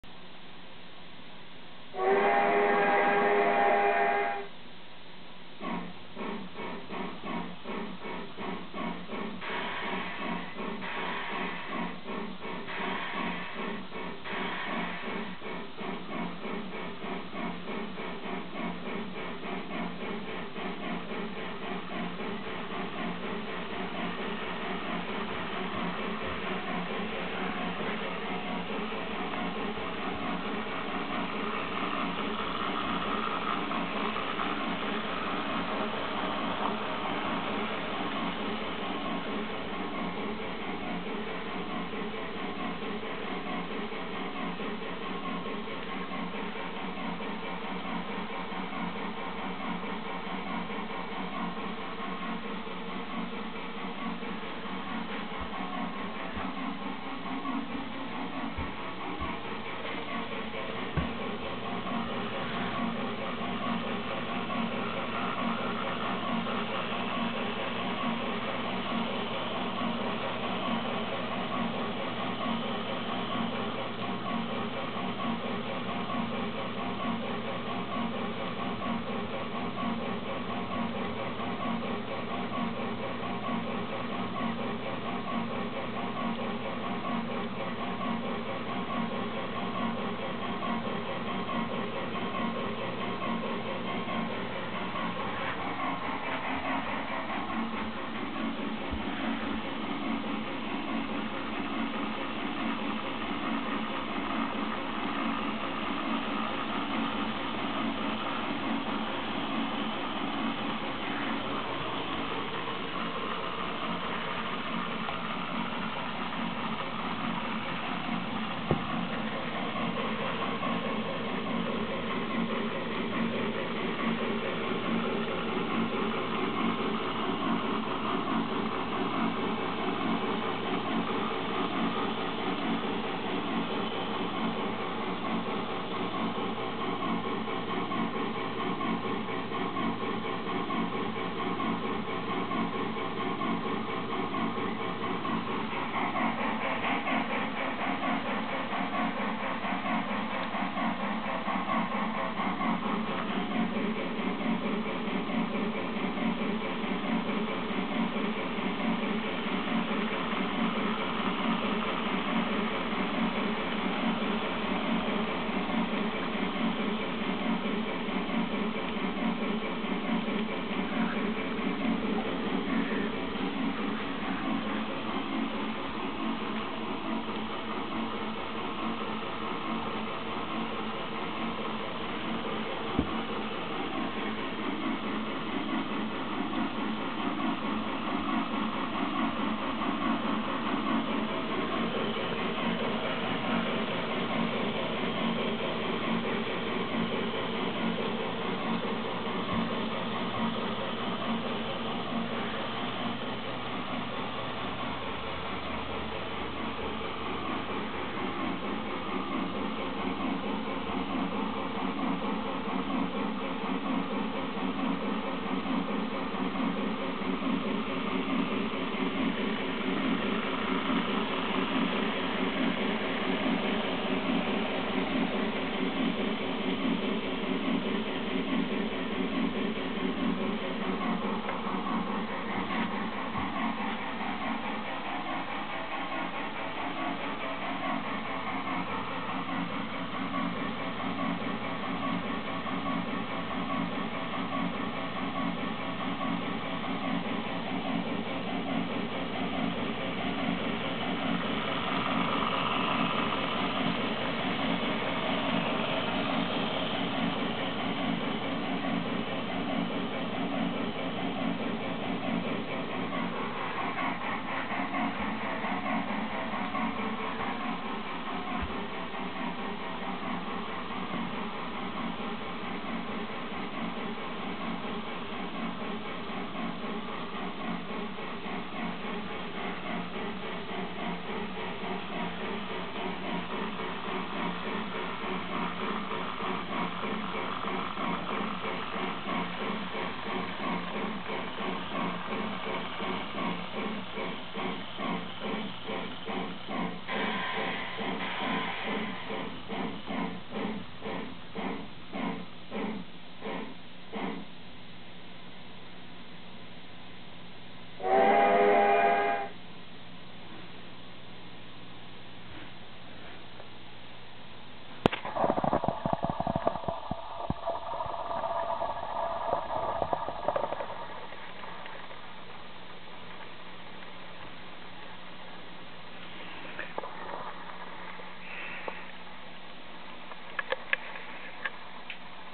HOお座敷レイアウトで重連走行① 米国Ｎ＆Ｗのﾏﾚｰ蒸気Y6b in Japan
HOゲージお座敷レイアウトで米国のＮ＆Ｗ 2-8-8-2 マレー式蒸気機関車 Y6bを重連走行させました。古いリバロッシ製です。貨車は約２０両、もう３０年以上前の車輌ですから、動きが遅いのですが、結構スケール感のある走行になりました。サウンドは実装でなく、MRCのサウンドシンクロボックスを２台使い、DC走行で重連のドラフトの感じを出しましたが、いかがでしょうか？中央丸型SPでサウンド再生しています。カメラワークが悪くてゴメンナサイ。